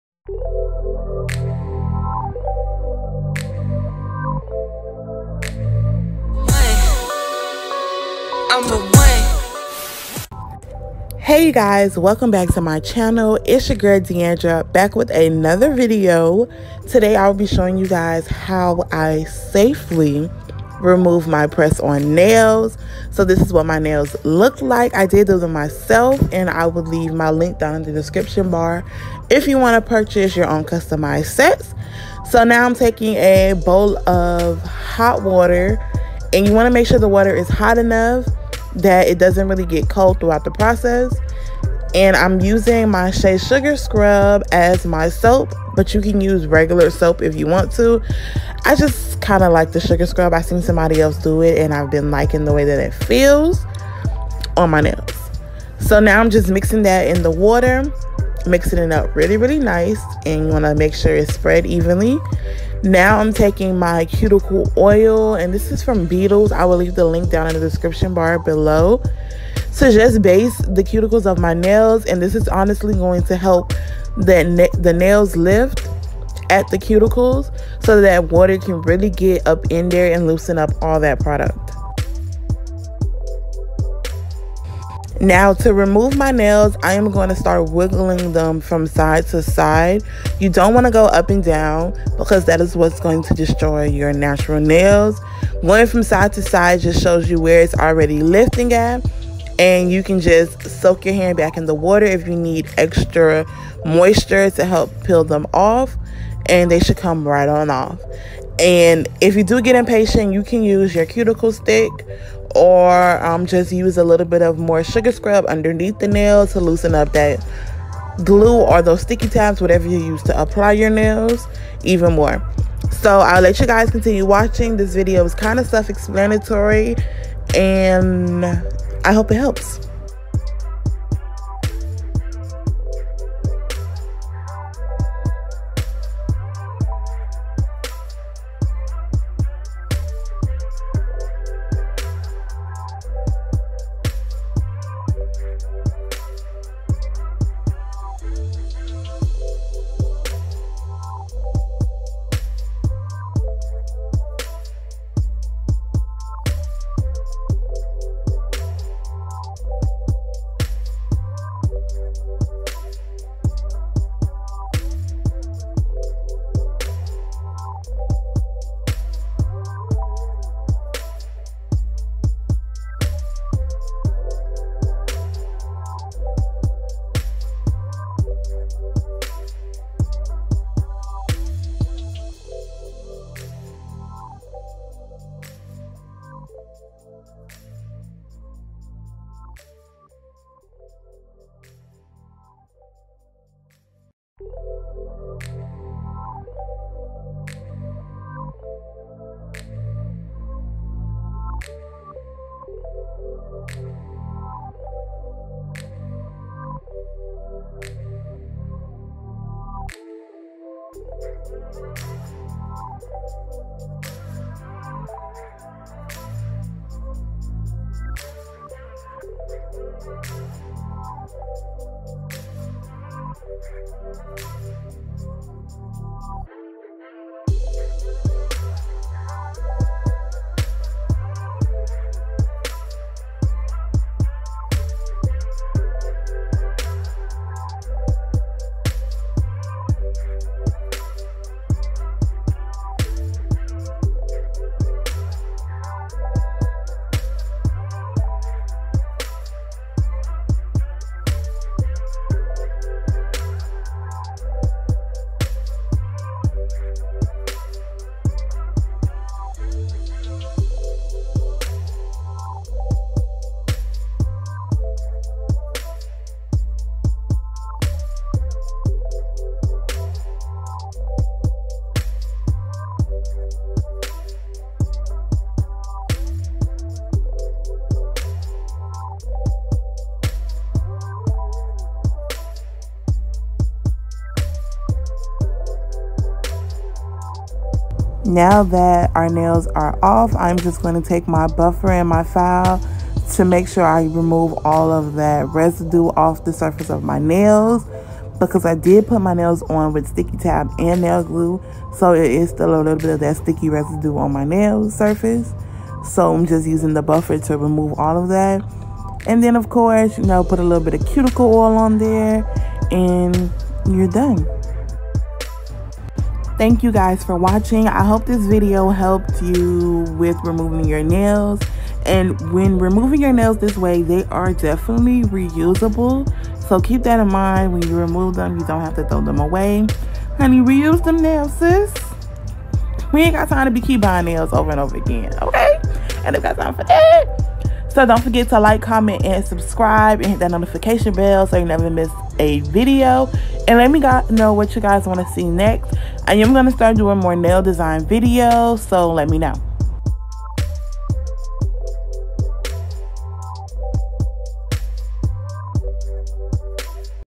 HOW TO: Remove Press On Nails WITHOUT Damage|D. Michelle Beauty
Lighting: Neewer 18" Ringlight